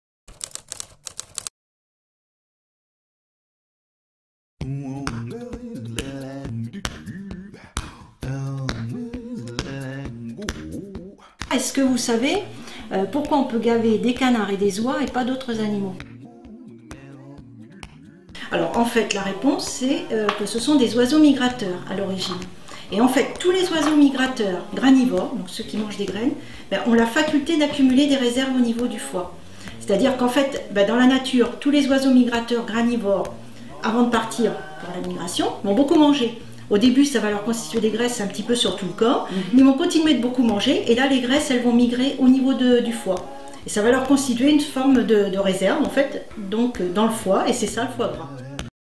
Comment faire le foie gras ?
Direction l'Aisne et plus précisément Condé-en-Brie pour un cours de cuisine pas comme les autres. A 1h30 de Paris, sur la route touristique du champagne, j'ai rendez-vous avec Fanny Fagot, propriétaire avec son mari des Canardises des 3 Vallées. Chez ce couple d'éleveurs et artisans, tout tourne autour du canard.